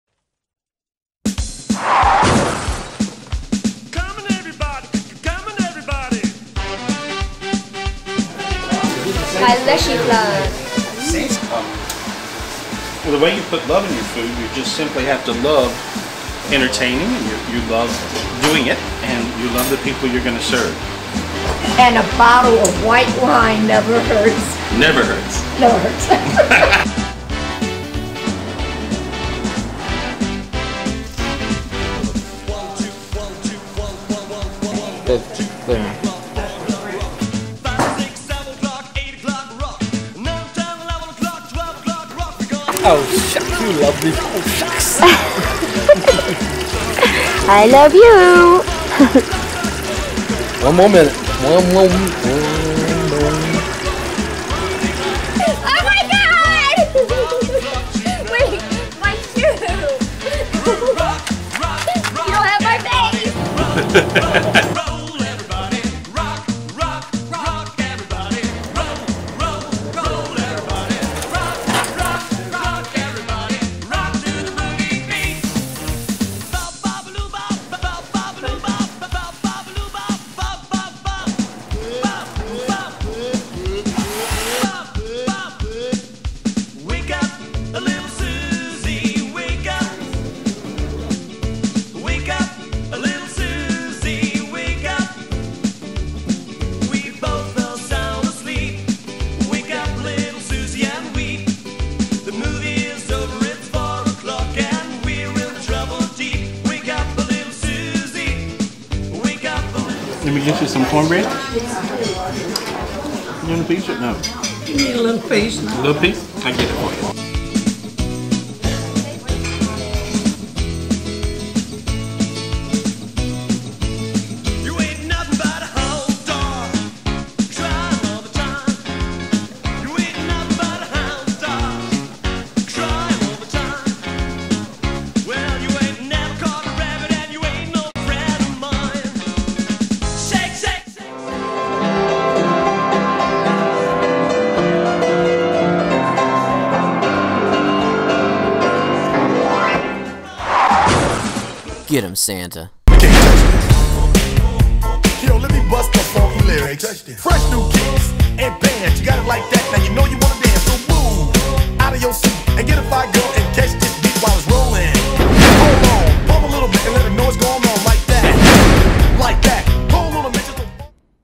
ThanksGiving Dinner at Gary Spivey's House Happy Holidays 2011
Hi guys, just want to wish everybody a very happy and blissful holiday season

LIKE GARY ON FACEBOOK!

TWEET GARY ON TWITTER!